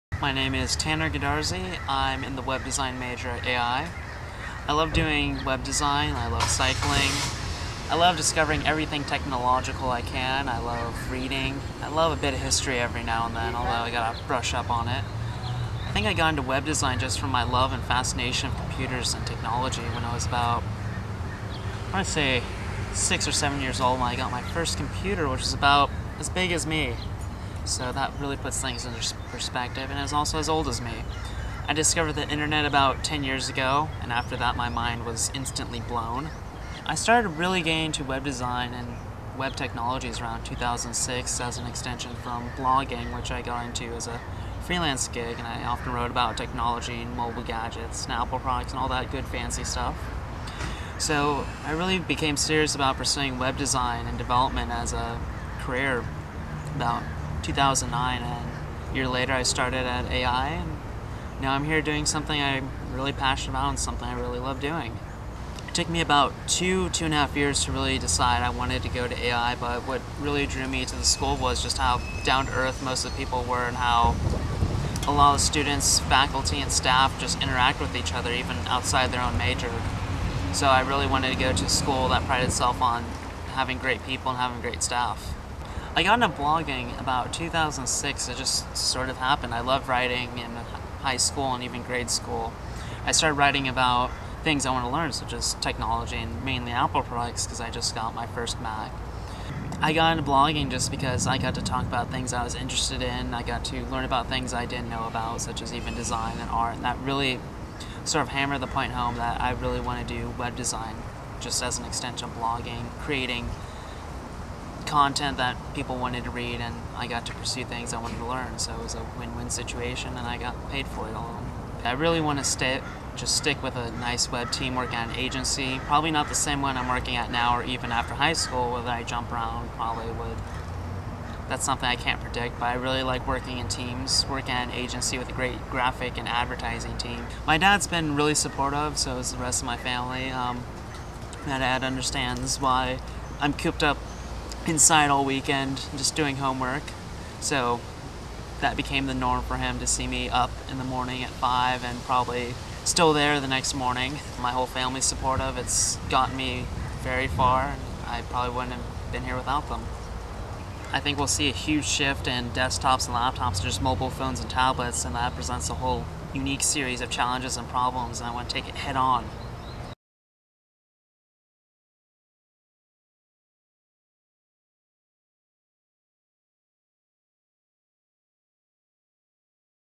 A Web Designer's Education
Tanner Godarzi is really pursuing his goal to become the best web designer at The Art Institute of O.C..